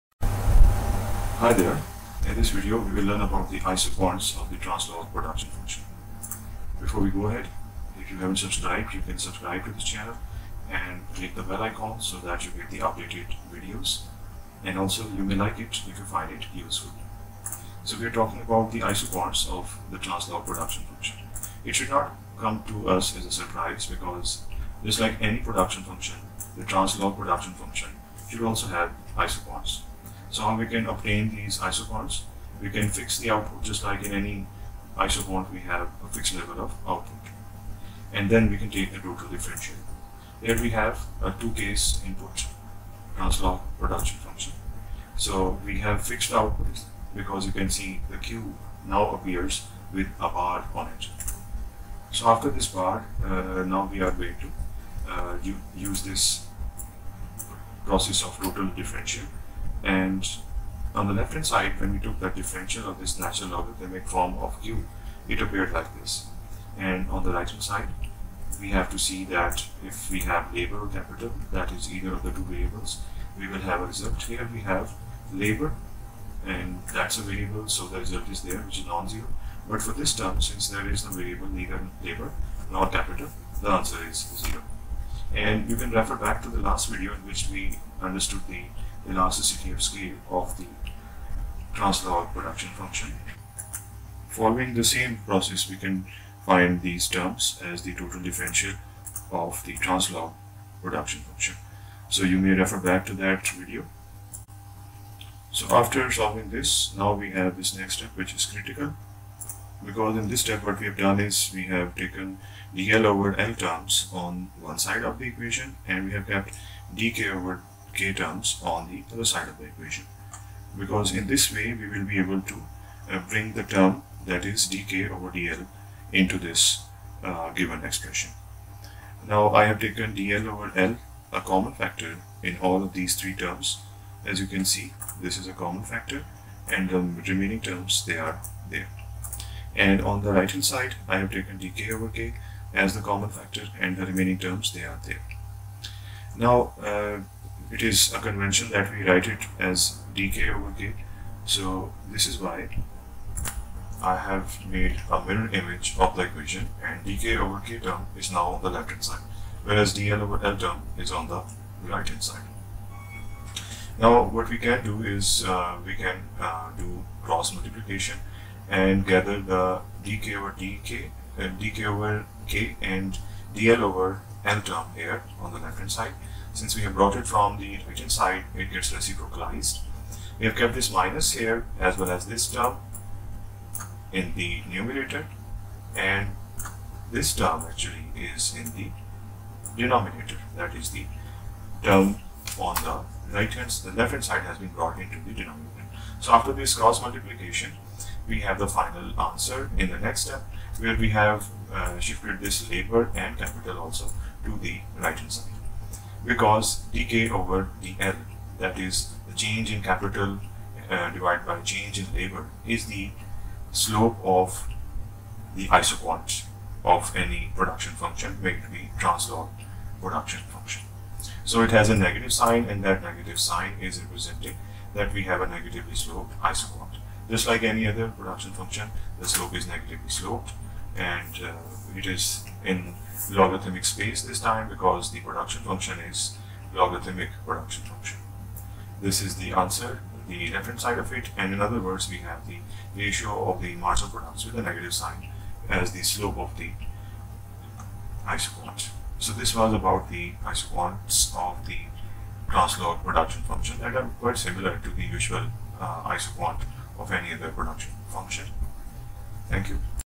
Isoquants of Translog Production Function
This video develops the Isoquants of Translog Production Function.

Regards,
DBM,